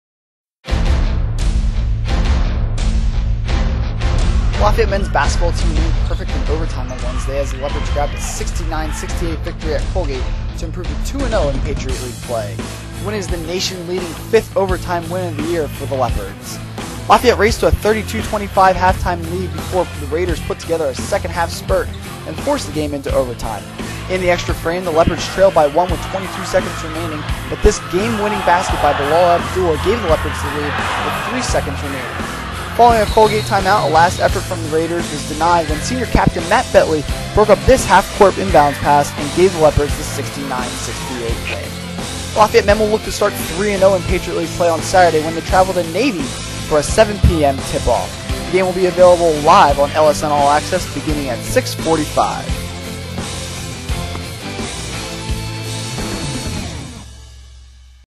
Lafayette Men's Basketball: Colgate Highlights
Lafayette improves to 2-0 in the Patriot League with a 69-68 OT victory at Colgate.